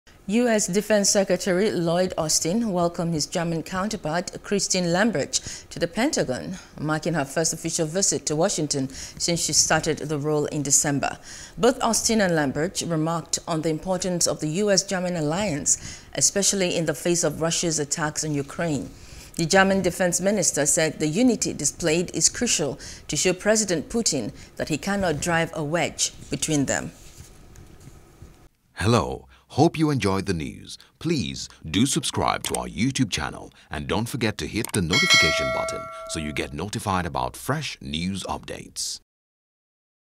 Russia-Ukraine War: German Defense Minister Visits Pentagon | FOREIGN
U.S. Defense Secretary Lloyd Austin welcomed his German counterpart, Christine Lambrecht, to the Pentagon, marking her first official visit to Washington since she started the role in December.